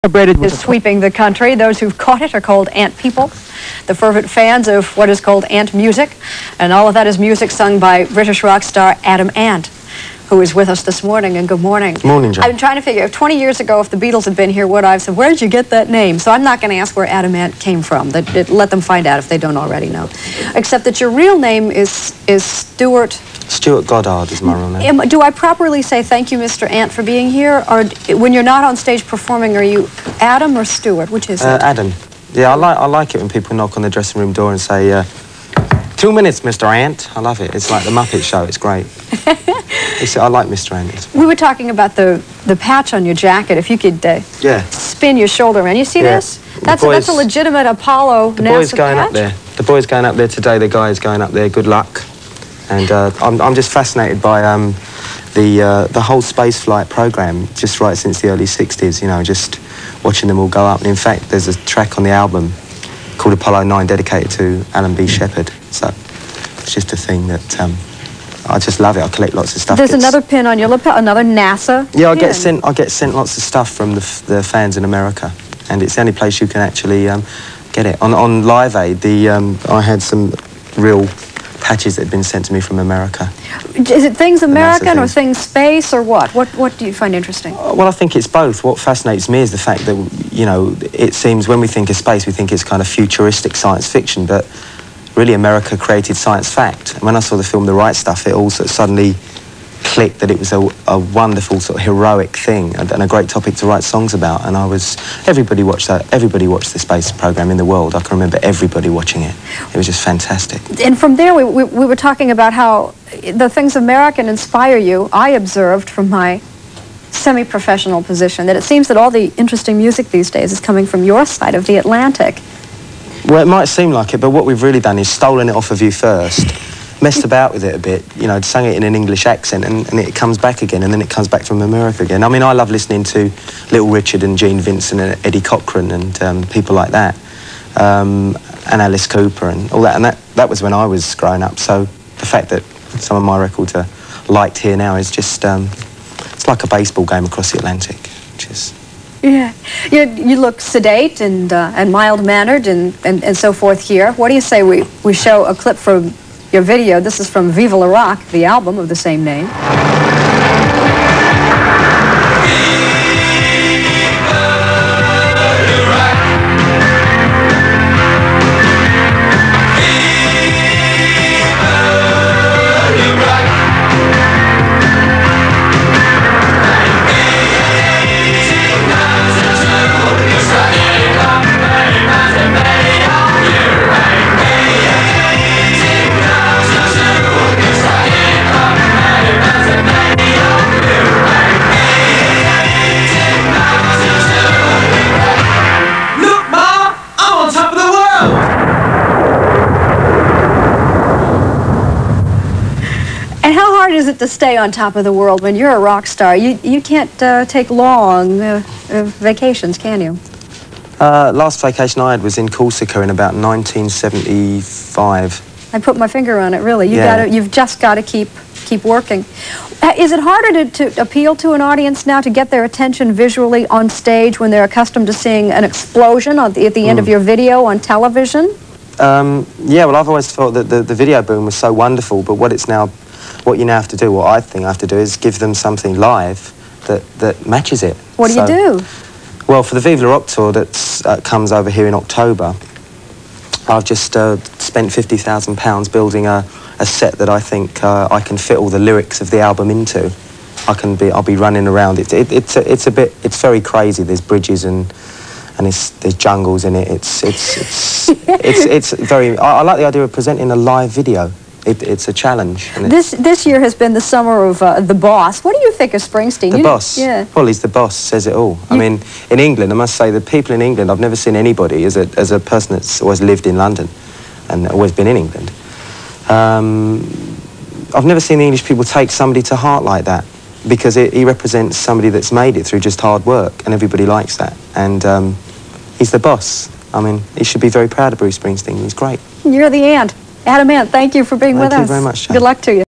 NBC's Jane Pauley Interviews Adam Ant (1985)
NBC's Jane Pauley interviewed Adam Ant prior to his 1985 "Viva Le Rock" US Tour.